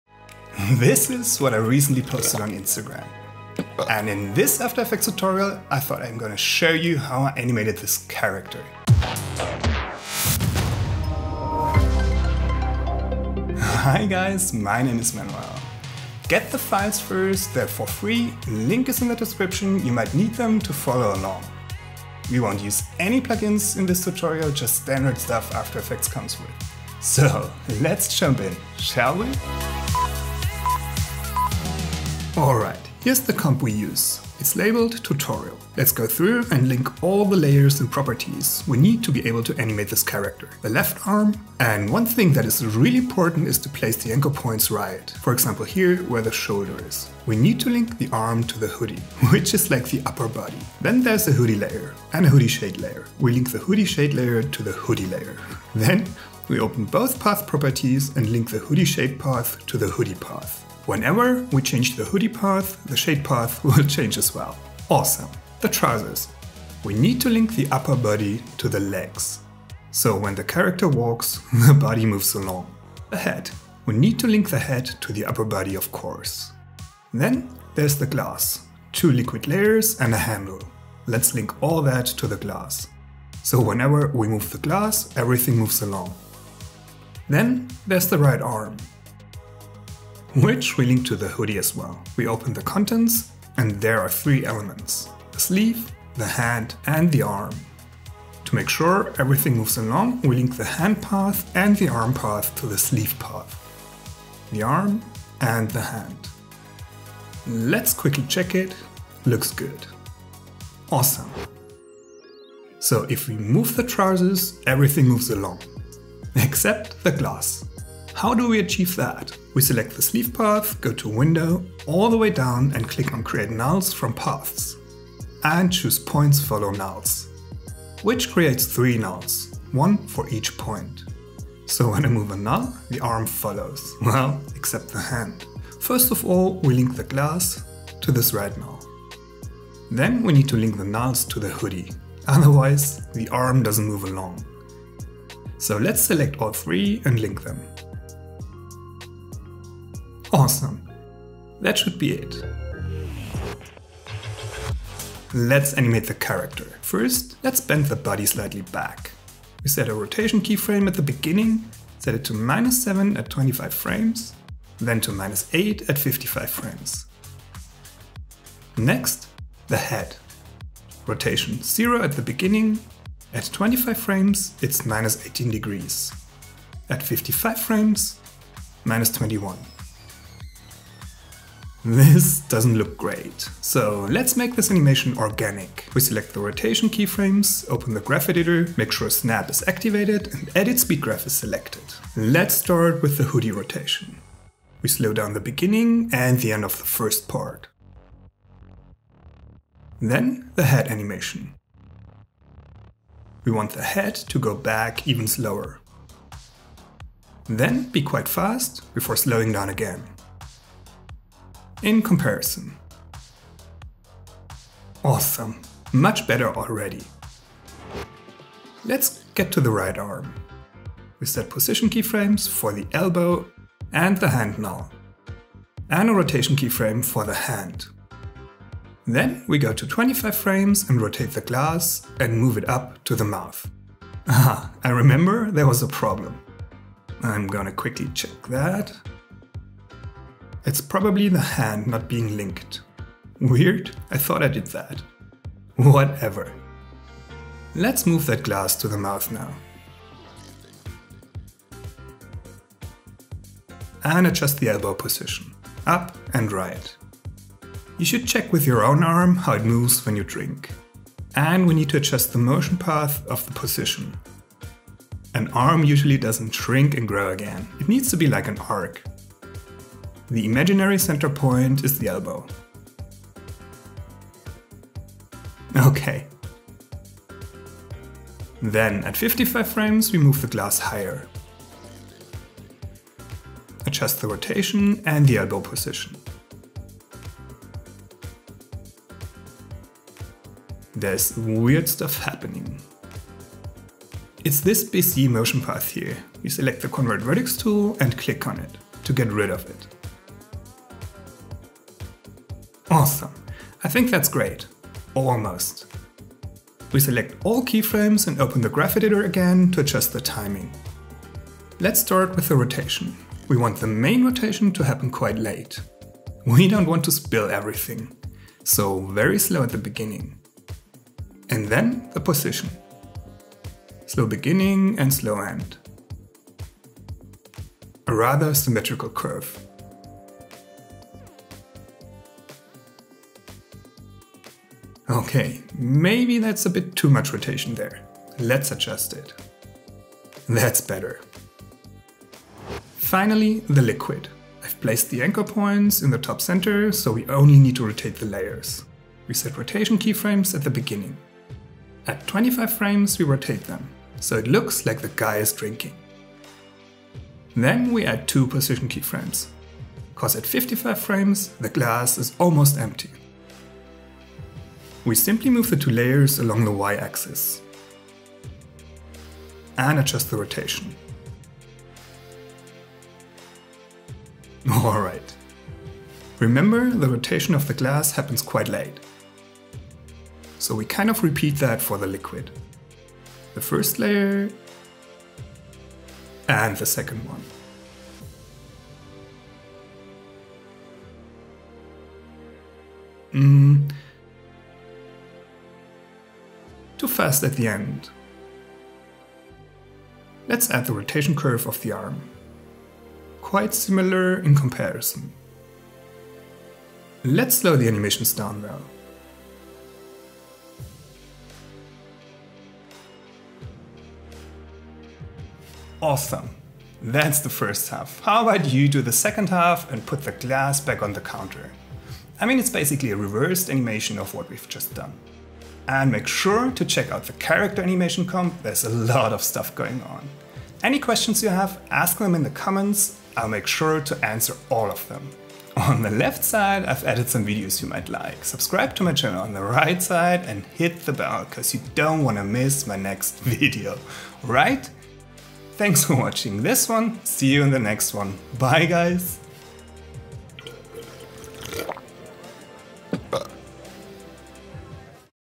Easy Character Animation in After Effects | Tutorial
In this After Effects Tutorial, I'll show you how to animate a character without using any third-party Plug-ins.

Content:
0:00 Intro
0:31 Linking layers and paths
1:58 Using the Create Nulls from Paths script
2:34 Animating the body and head
3:38 Animating the arm and the glass
5:42 Animating the liquid
6:58 Outro